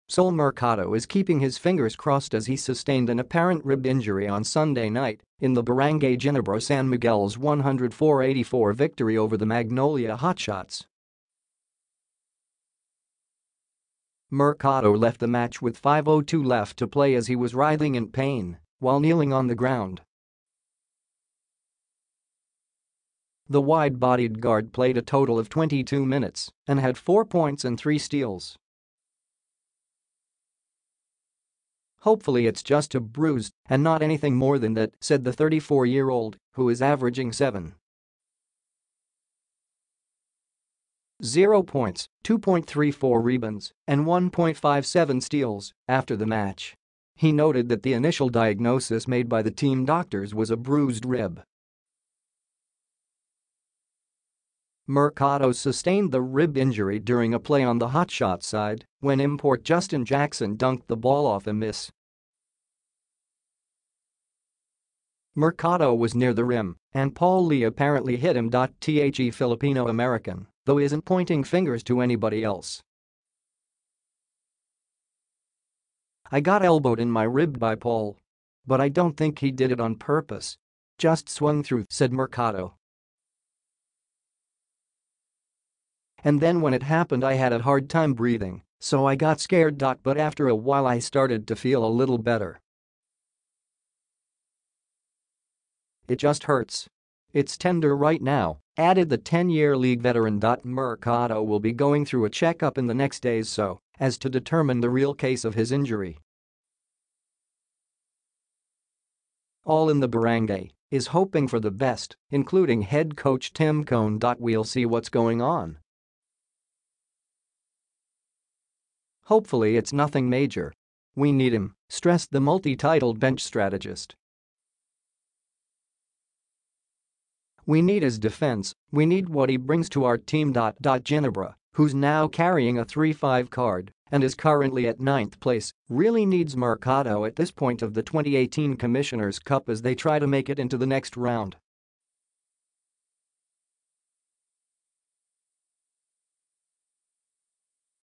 Sol Mercado hopes rib injury won't be something threatening
Sol Mercado is keeping his fingers crossed as he sustained an apparent rib injury on Sunday night, in the Barangay Ginebra San Miguel’s 104-84 victory over the Magnolia Hotshots.

Mercado left the match with 5:02 left to play as he was writhing in pain while kneeling on the ground. The wide-bodied guard played a total of 22 minutes and had four points and three steals.

“Hopefully it’s just a bruise and not anything more than that,” said the 34-year-old, who is averaging 7.0 points, 2.34 rebounds, and 1.57 steals, after the match. He noted that the initial diagnosis made by the team doctors was a bruised rib.

Mercado sustained the rib injury during a play on the Hotshots side, when import Justin Jackson dunked the ball off a miss. Mercado was near the rim, and Paul Lee apparently hit him.

The Filipino-American though isn’t pointing fingers to anybody else

“I got elbowed in my rib [by] Paul. But I don’t think he did it on purpose. Just swung through,” said Mercado. “And then when it happened I had a hard time breathing, so I got scared.

“But after a while I started to feel a little better. It just hurts. It’s tender right now,” added the 10-year league veteran.

Mercado will be going through a check-up in the next days so as to determine the real case of his injury. All in the Barangay is hoping for the best, including head coach Tim Cone.

“We’ll see what’s going on. Hopefully it’s nothing major. We need him,” stressed the multi-titled bench strategist. “We need his defense, we need what he brings to our team.”

Ginebra, who’s now carrying a 3-5 card and is currently at ninth place, really needs Mercado at this point of the 2018 Commissioner’s Cup as they try to make it into the next round.